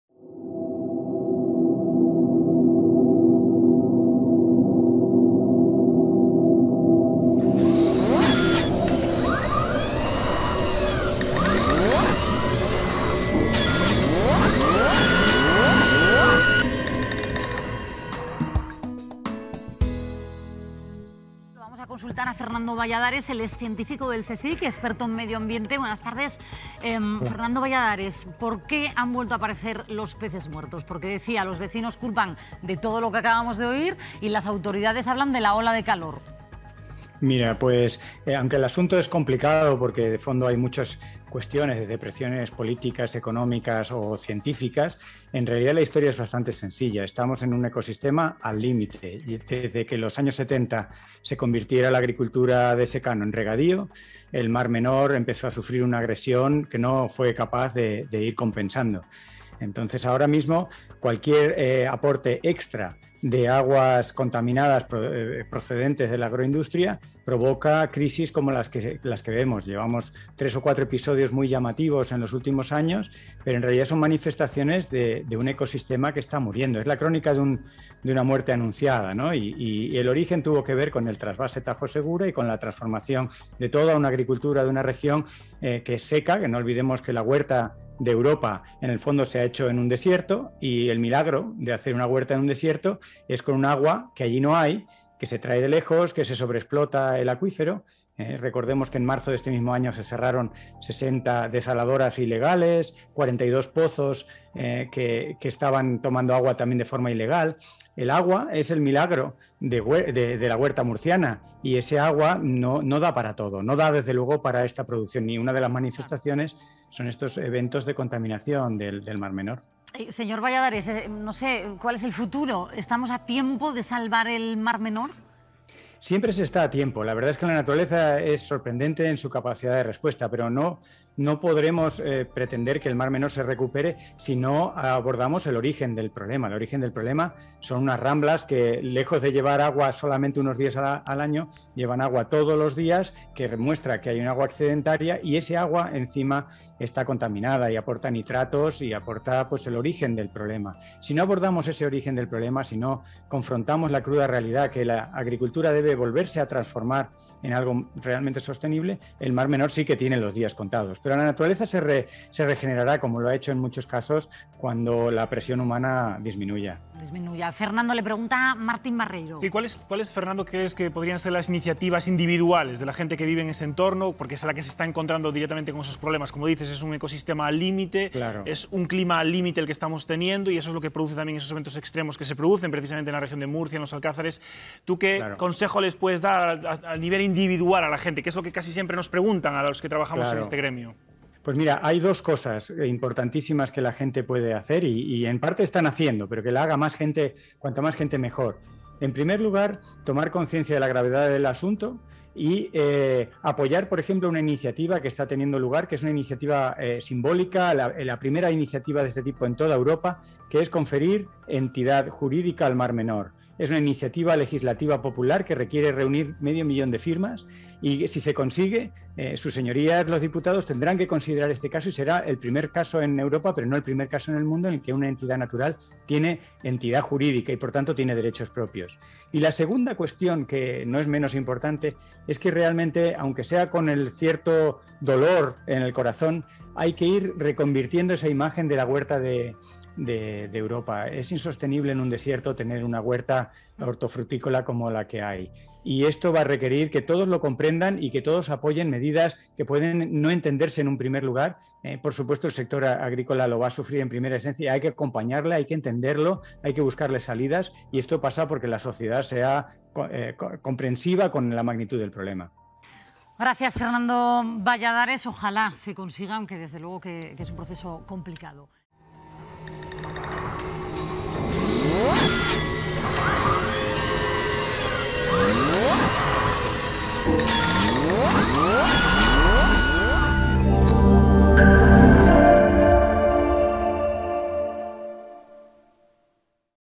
51 - La tragedia del Mar Menor
El Mar Menor fue uno de los espacios naturales más importantes de Europa y el lugar de veraneo preferido por muchos españoles. Hoy es un monumento al desastre ambiental que sale periódicamente en las noticias por eventos agudos de contaminación acompañados de imágenes impactantes de miles de peces muertos. Son el brutal desenlace de tres décadas de incumplir la ley. Tan sucio y maloliente está el Mar Menor que mucha gente lo da por perdido. O, lo que es aún peor, lo considera un sitio adecuado para verter desperdicios y aguas contaminadas. Tan turbia está la escena social y política en Murcia, que muchos agricultores creen que su subsistencia depende de seguir contaminando el Mar Menor y extrayendo sin límite el agua del subsuelo o del trasvase Tajo-Segura, cuando es precisamente eso lo que compromete su subsistencia.